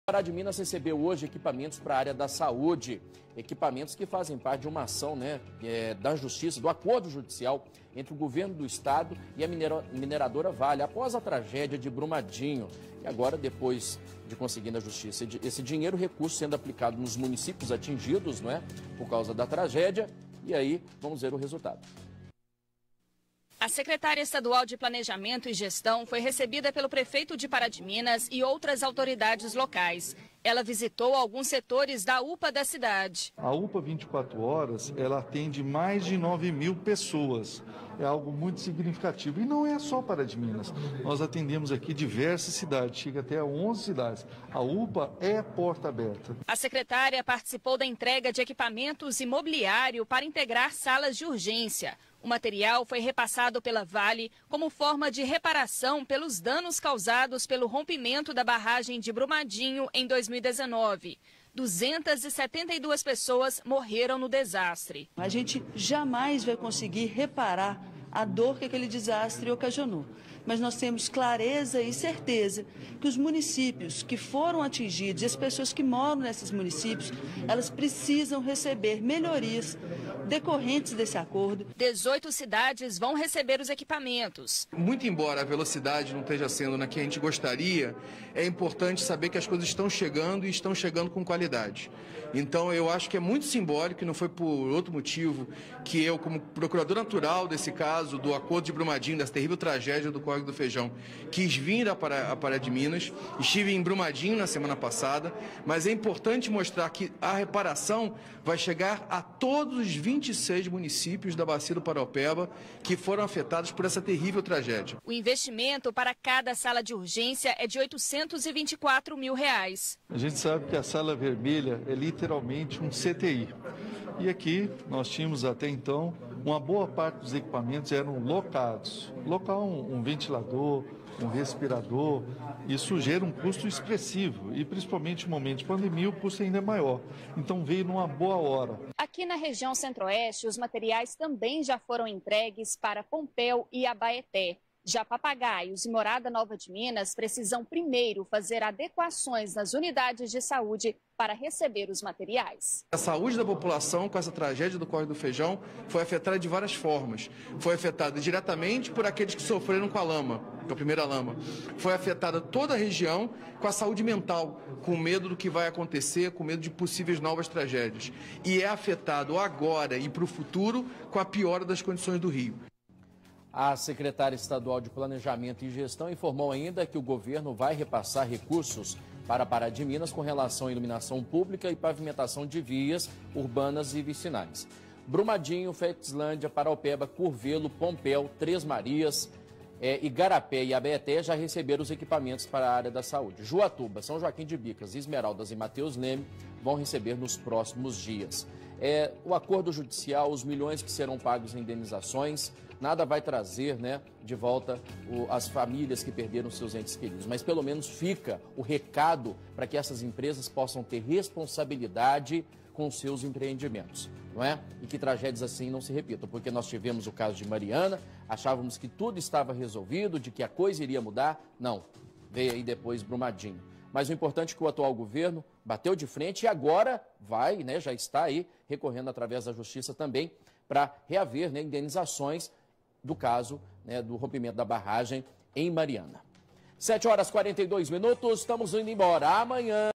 UPA recebe equipamentos de saúde
Ação que faz parte do acordo judicial entre o Governo do Estado e Mineradora Vale, após a tragédia de Brumadinho.